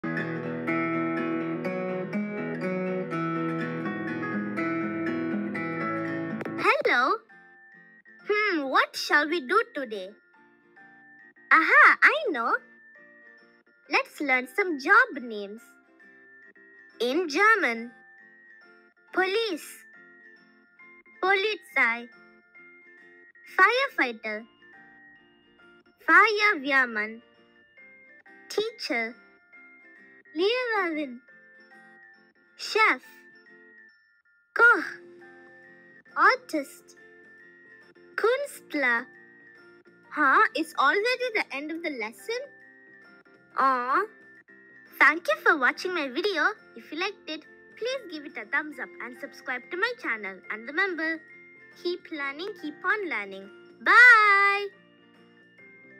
Lets learn some Job Names In German - Learning Languages by Languages With Nila
Hello my dear friends.Today, We are going to learn some Job Names In German with Mishel .Let's get started!